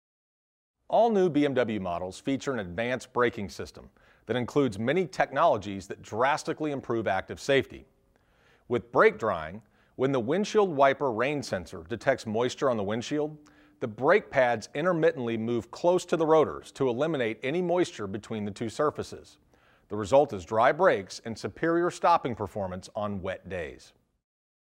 BMW Brake Drying
Kuni BMW | In this video, learn how the brake drying function in a BMW provides dry brakes and superior stopping performance on wet days.